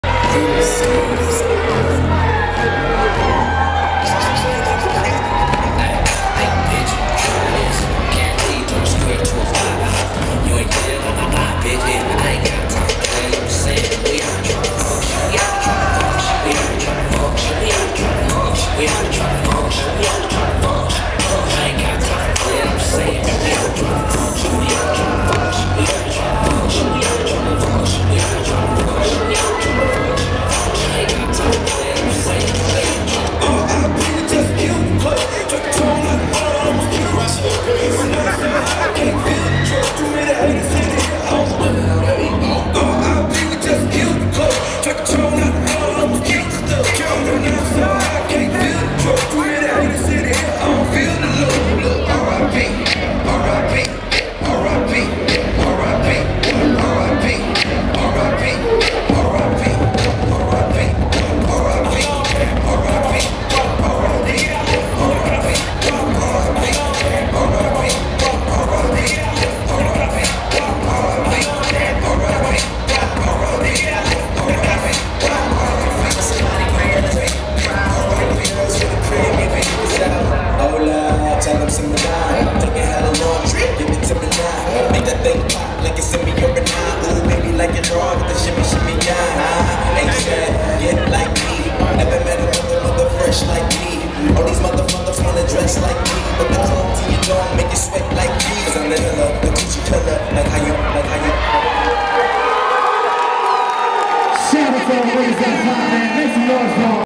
Greek stroll off
PHI PSI Chapter of Omega Psi Phi inc. setting it OWT! ROO!